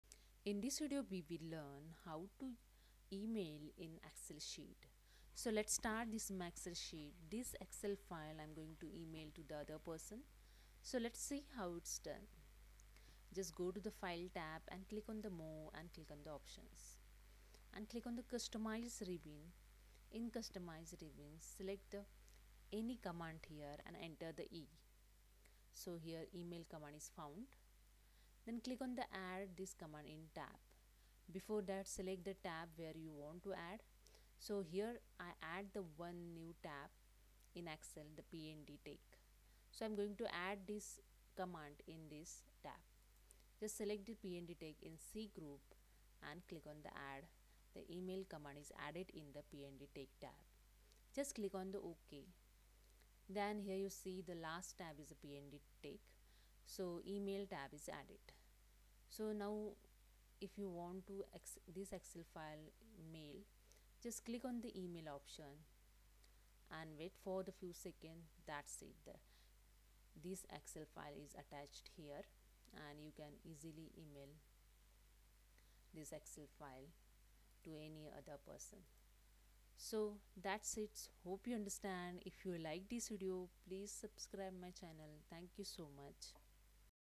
How To Email In Excel
Send as an attachment
Click File Option - Email, and then choose one of the following options: Send as Attachment Opens an email message with a copy of the file in its original file format attached. ...
Enter the recipients' aliases, edit the subject line and message body as necessary, and then click Send.